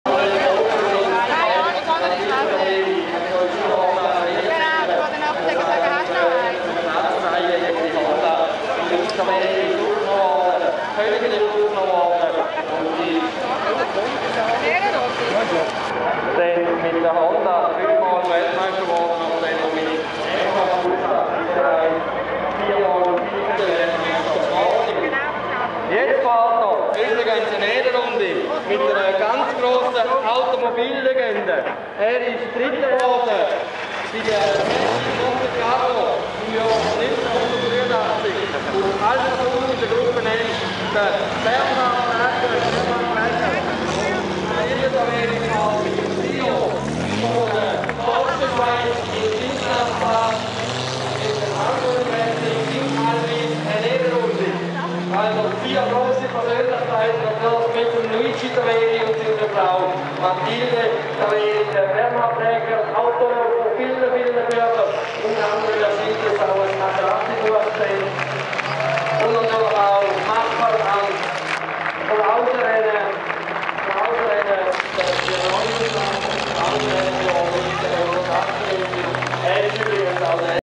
Luigi Taveri mit Frau auf Ehrenrunde am Indianapolis in Oerlikon 2012
Bye Bye Luigi Taveri